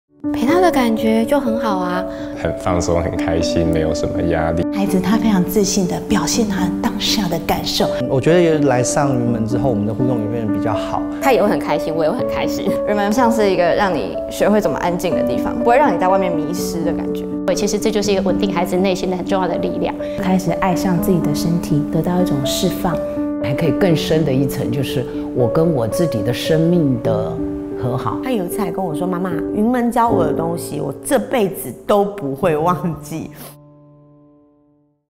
在雲門教室學會的，一輩子也不會忘
在雲門教室，你會感覺每一個人自己都有很鮮明的色彩，好像會發光，但這麼多顏色組合在一起，卻讓人覺得舒服，自在，快樂。

──幸薇，生活律動14年